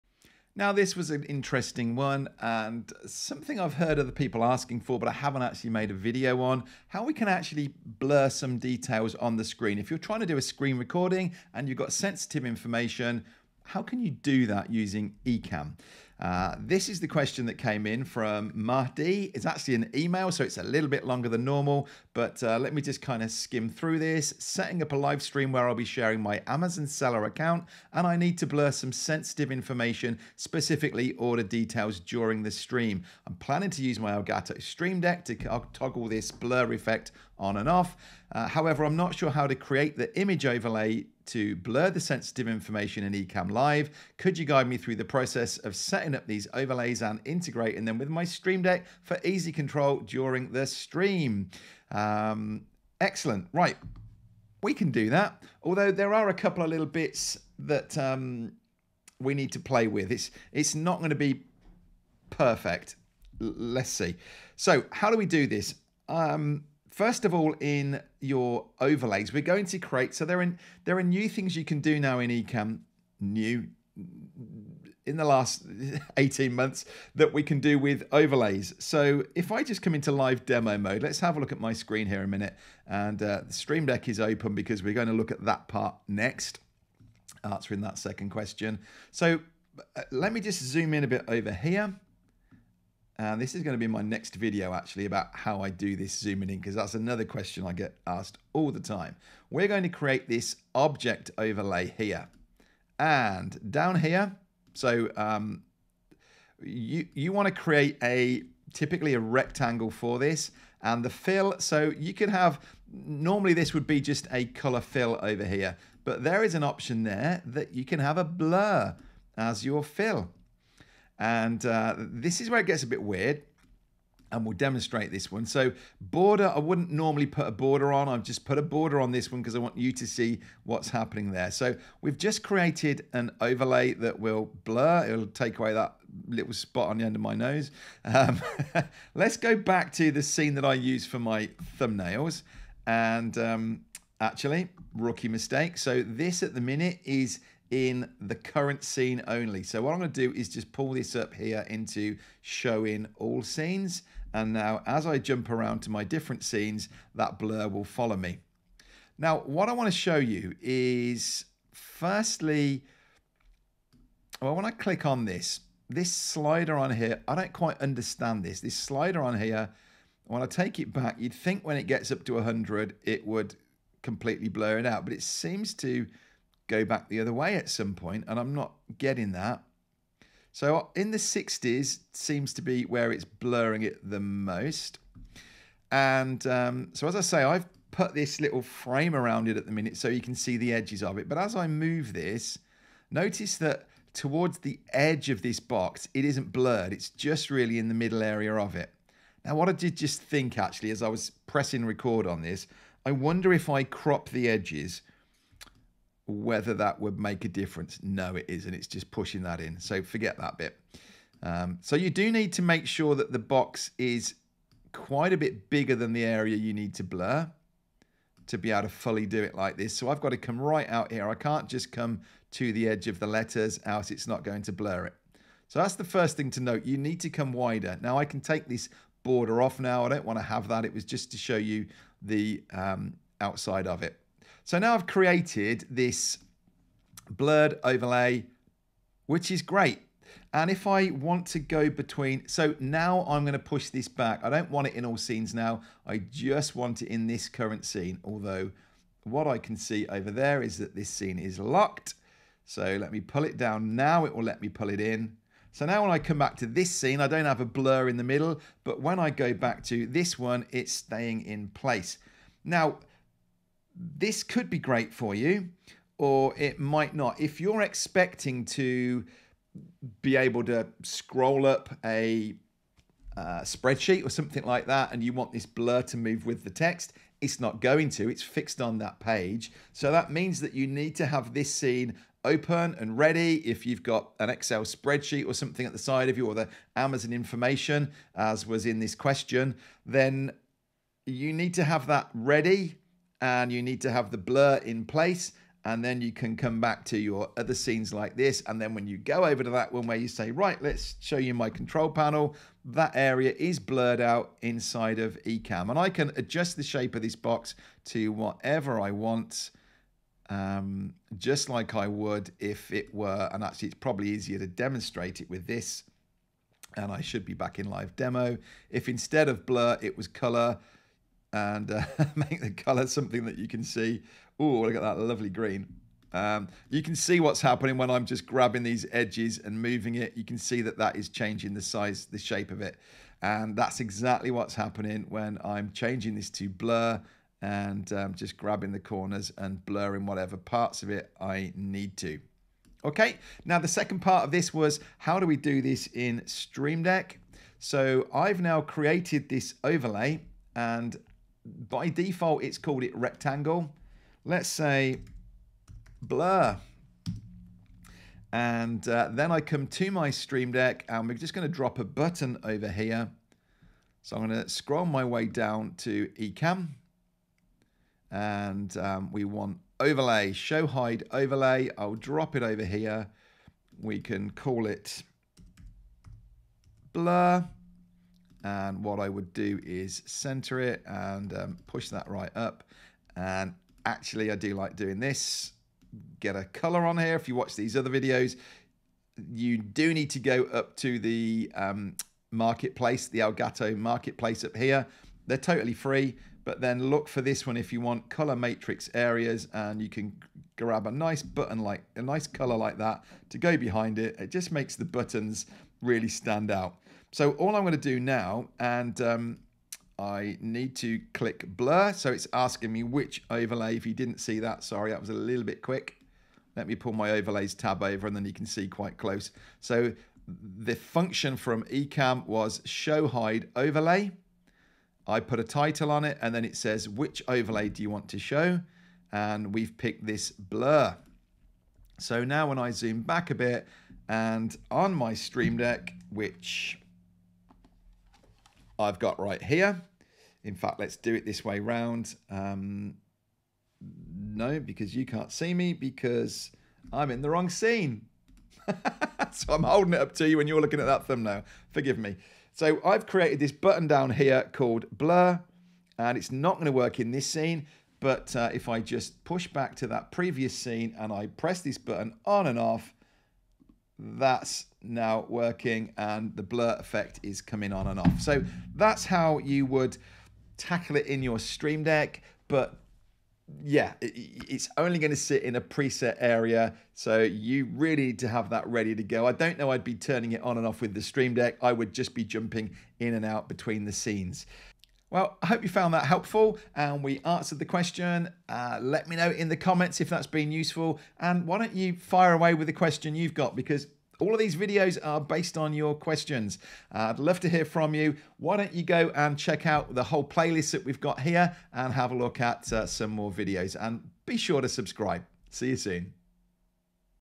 How To Blur a Screen Share in Ecamm Live
In this video I answer the question 'do you need a new streamdeck profile for each ecamm profile?' Plus I give some PRO tips for using the Streamdeck 🤓